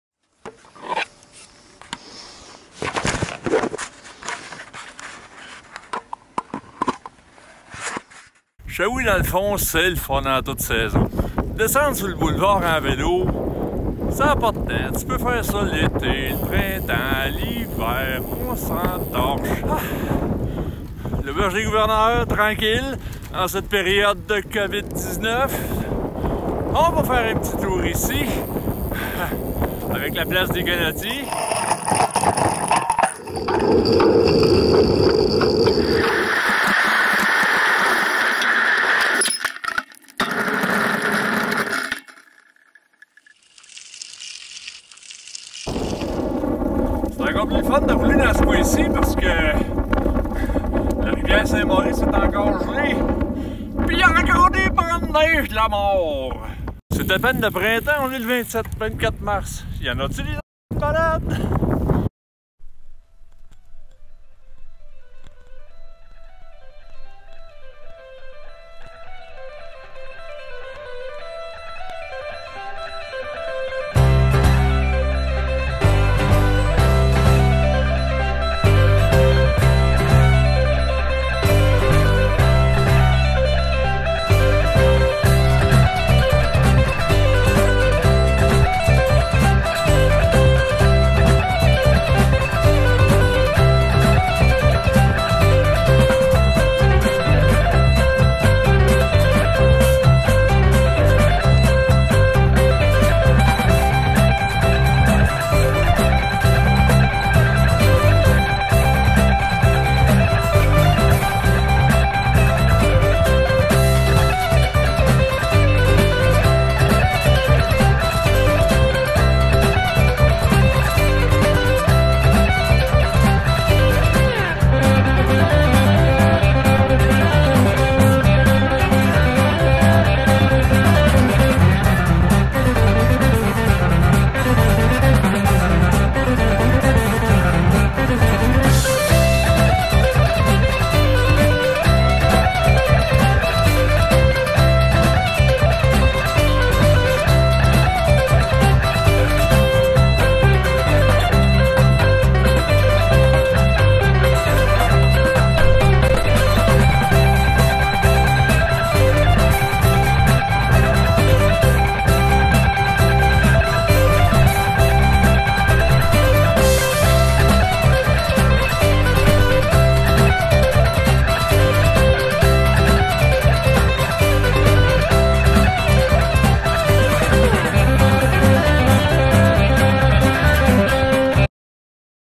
Un cycliste combat la déprime de la COVID-19
Décrochez des nouvelles et de la déprime. Roulez à au moins deux mètres de distance... des automobilistes :-) Une petite virée dans les rues de Shawinigan en ce beau 24 mars 2020